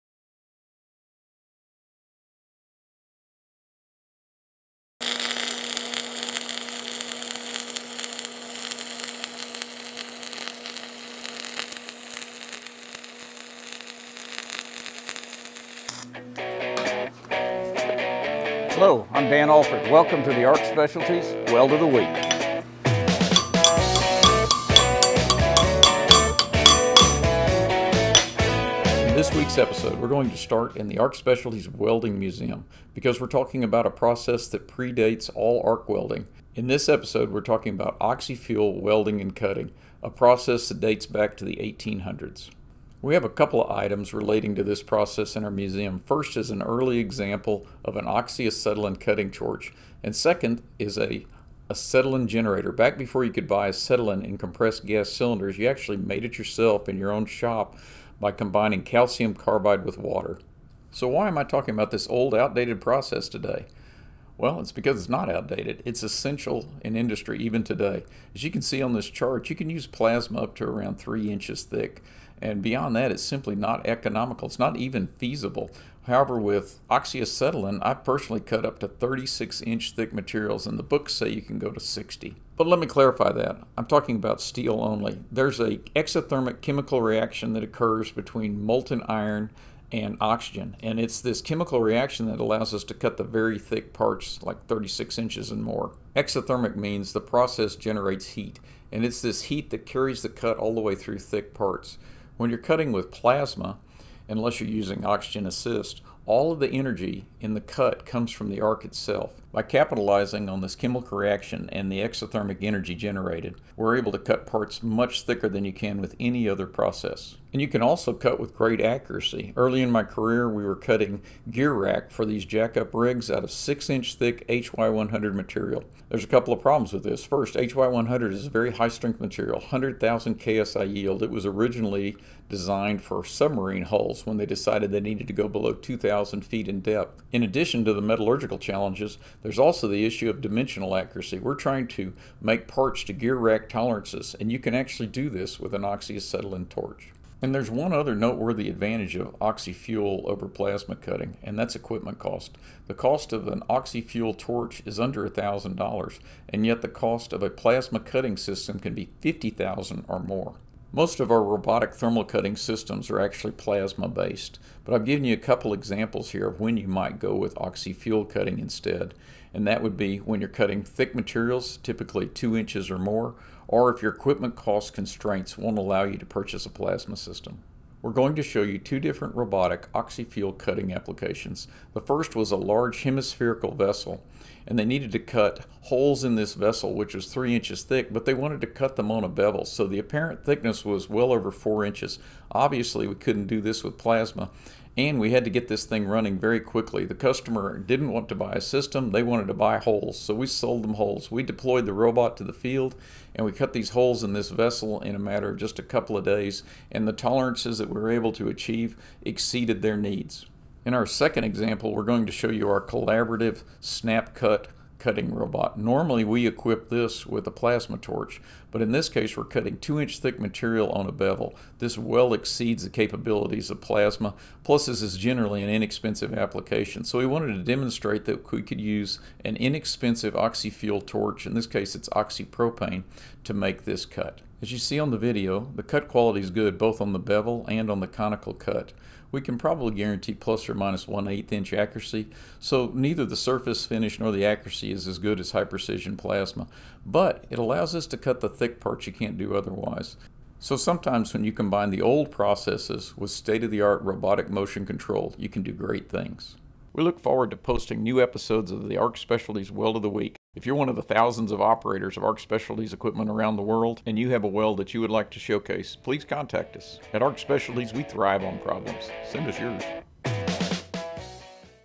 WOW Episode 11: Robotic Oxy-Fuel Cutting
At ARC Specialties most of our robotic cutting systems are based on plasma. But plasma is limited on part thickness. My rule of thumb is you need 100 amps of power per inch of thickness. There are a few systems that can cut up to 4" but in my opinion 2" is the practical limit for economical plasma cutting.

However, for steel parts, there is another way.

Oxy-Fuel cutting was developed in the 1800s but it is still an essential industrial technology. It is limited to steel and occasionally titanium. The exothermic reaction of oxygen and the molten metal creates a heat source independent of the flame. This greatly increases cut thickness capacity. I have cut up to 36" steel to rough machining dimensions and the books say that 60" is possible. Plus a cutting torch is less than $500 which is 1% of the cost of a large plasma cutting system.

Today we showcase a couple of robotic oxy-fuel projects. If you need to cut heavy steel or your budget won't allow plasma send us some test parts and we will determine if robotic oxy-fuel will solve your 3-dimensional cutting problem.

Sometimes if you combine old technology with state-of-the-art robotic motion control you can do great things!

- Dan Allford